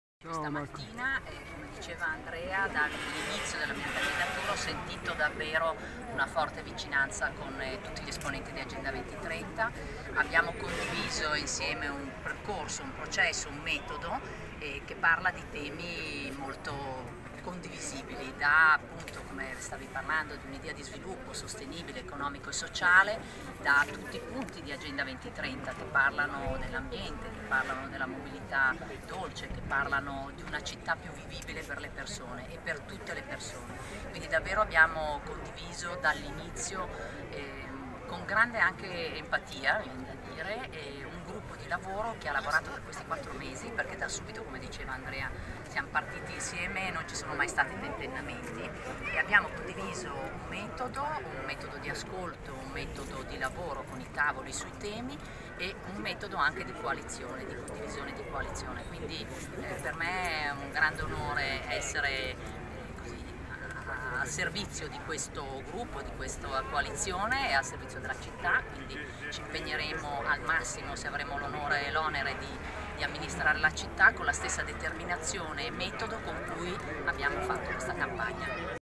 BARBARA MINGHETTI E LA PRESENTAZIONE DELLA LISTA AGENDA COMO 2030:"GRANDE LAVORO IN SINERGIA"
Le sue parole oggi in piazza Volta, presenti tutti i candidati consiglieri